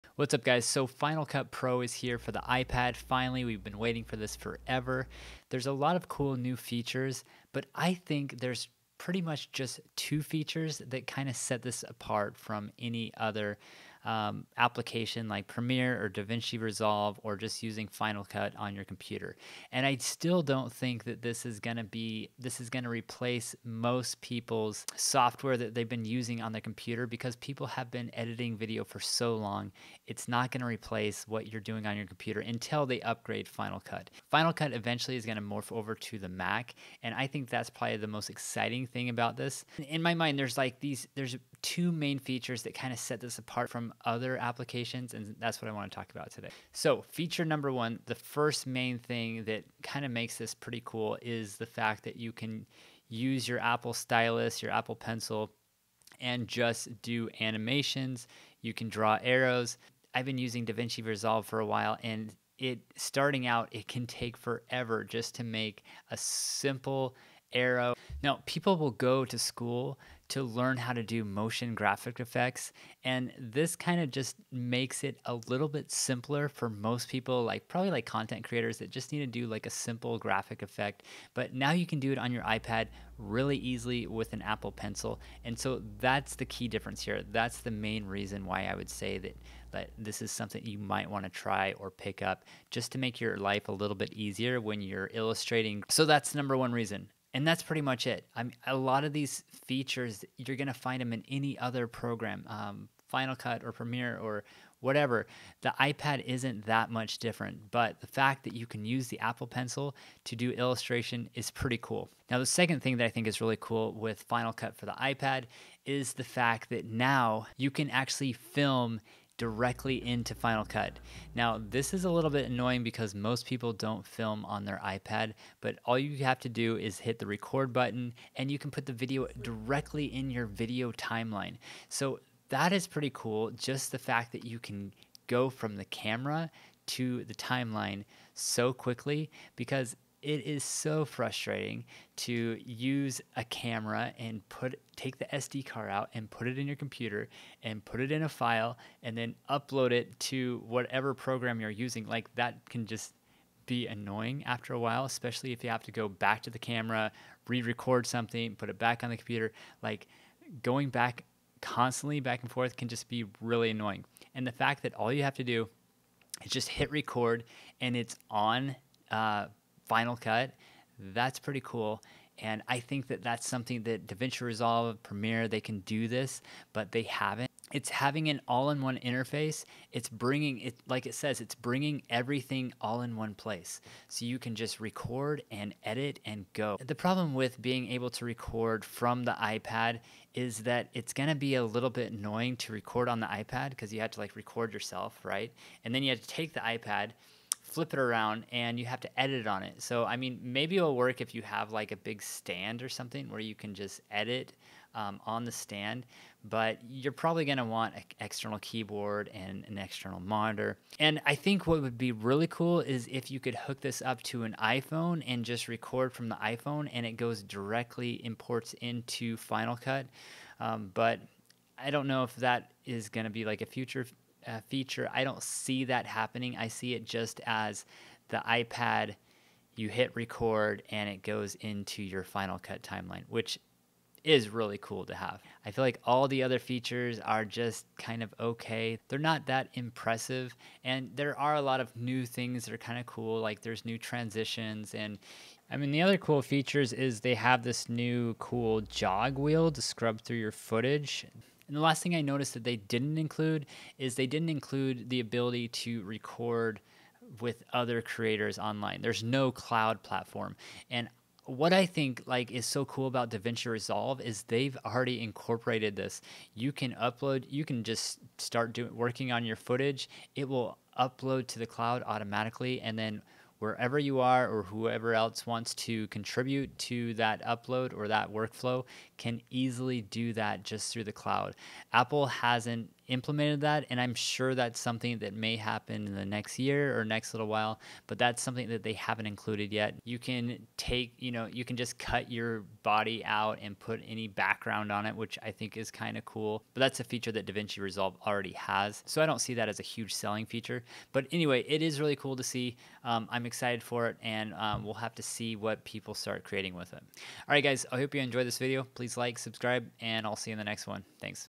Final Cut for iPad makes motion graphics and editing faster
In this video, I talk about the best new features for Final Cut Pro for the iPad and compare them to the features that are already offered by Davinci Resolve and Premiere Pro.

YouTube Gear (Links to Amazon)
► Main Cam
► Main Lens